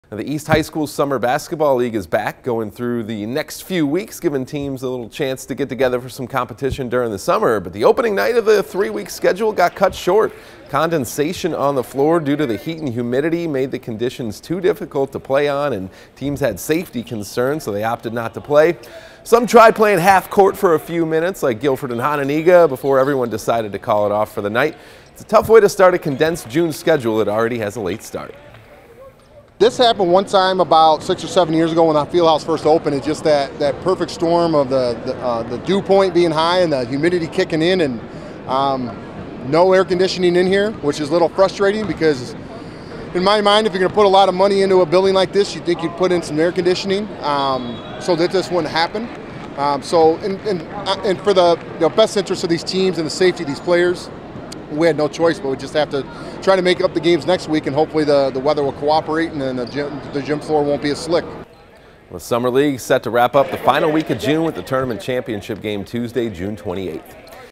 East Summer League cut short on opening night
Slippery conditions led to the postponement of games.